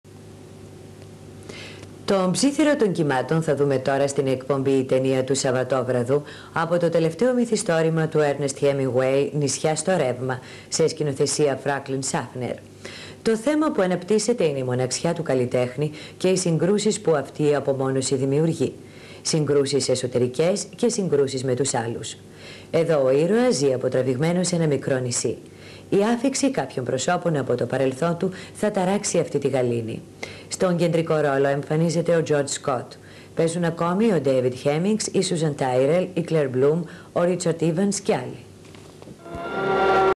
ΚΕΛΛΥ ΣΑΚΑΚΟΥ 1984
Αναγγελία προγράμματος από την τηλεπαρουσιάστρια της ΕΡΤ Κέλλυ Σακάκου.